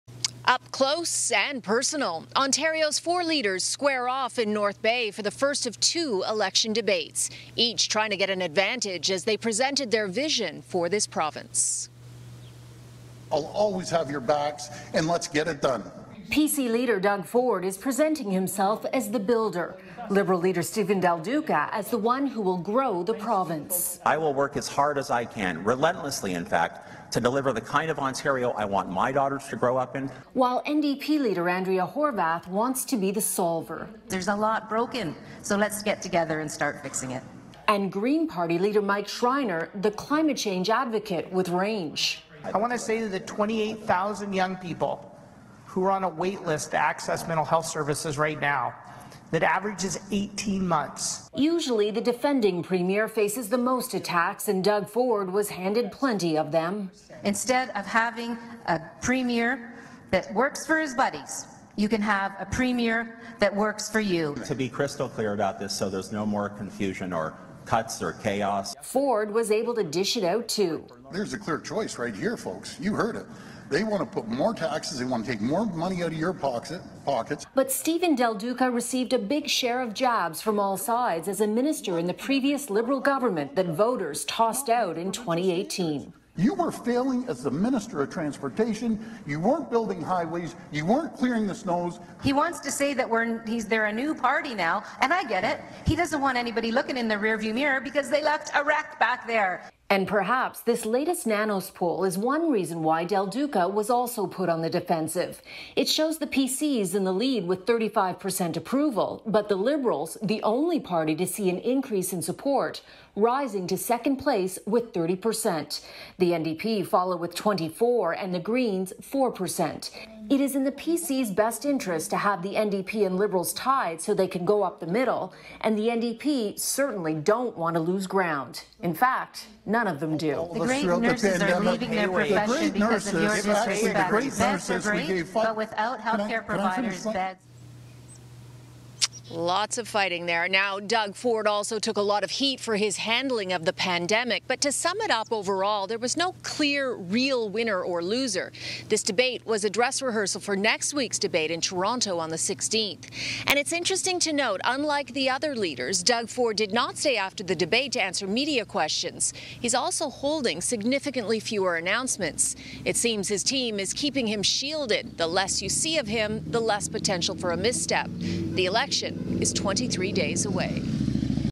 Ontario Party Leaders hold election debate in North Bay - May 10, 2022
On May 10, 2020, CityNews reports on the first of the 2022 provincial election debates held amongst party leaders Progressive Conservative Doug Ford, Liberal Steven Del Duca, the NDP's  Andrea Horwath, and the Green's Mike Schreiner.  This debate was held in North Bay.